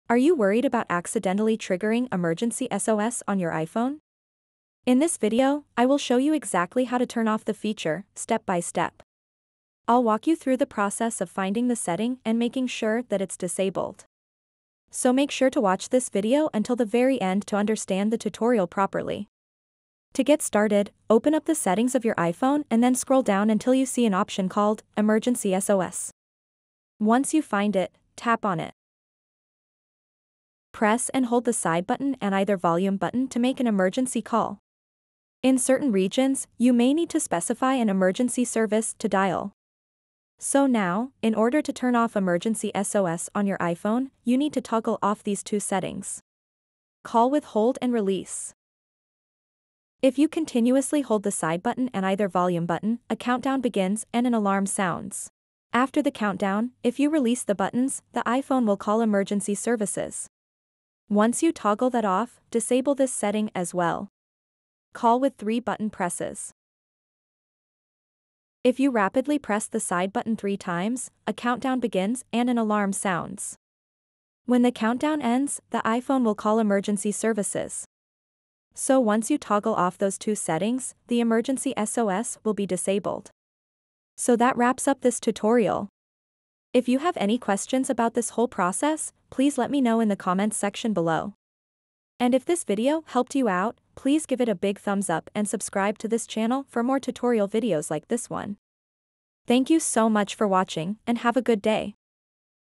How To Turn Off Emergency SOS On iPhone
Are you tired of accidentally triggering the Emergency SOS feature on your iPhone? Well, worry no more! This video will show you how to turn off Emergency SOS so that it won't get in your way. We'll walk through all the steps necessary to make sure this feature is disabled and out of sight. So if you're ready to learn how to turn off Emergency SOS on your iPhone, then watch this video now!